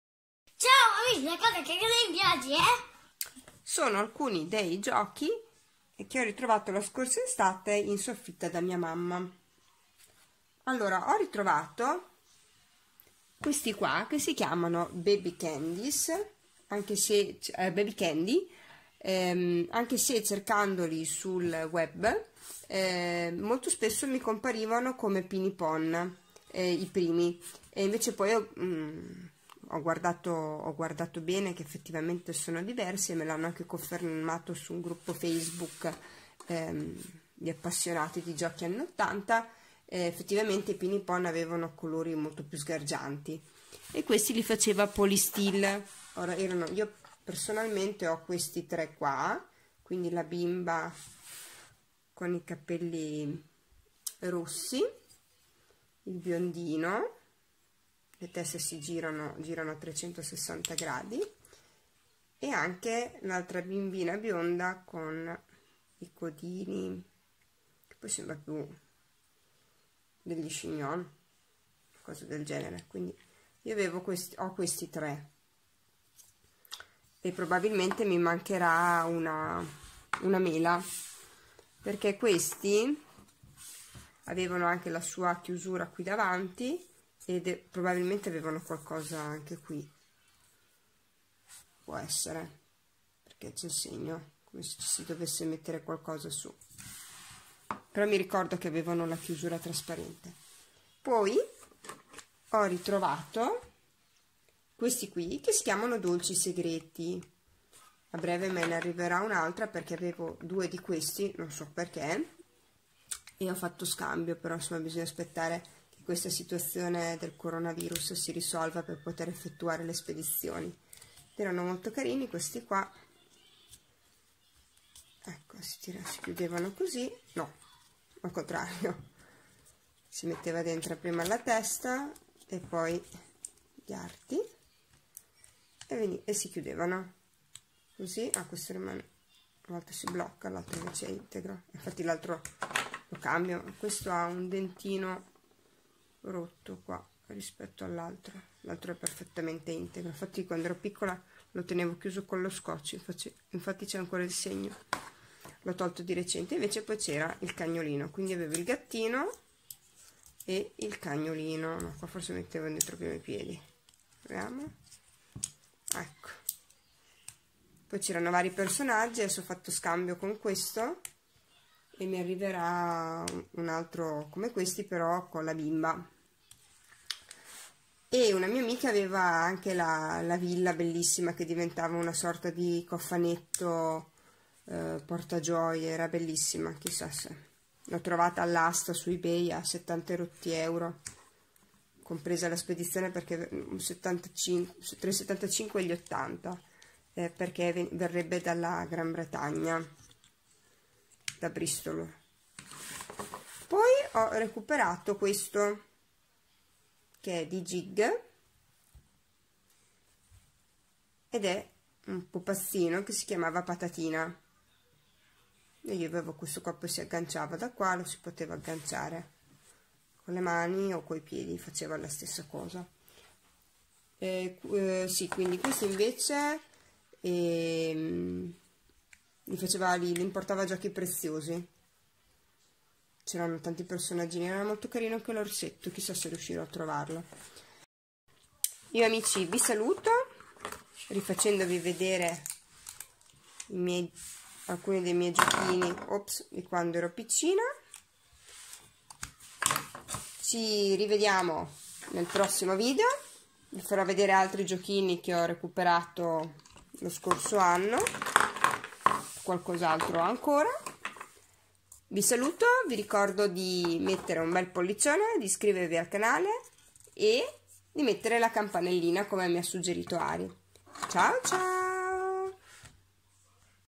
La cosa coi cosini di oggi è...giochini anni '80
Alcuni giochi della mia infanzia...Dolci segreti, Patatina, Baby Candy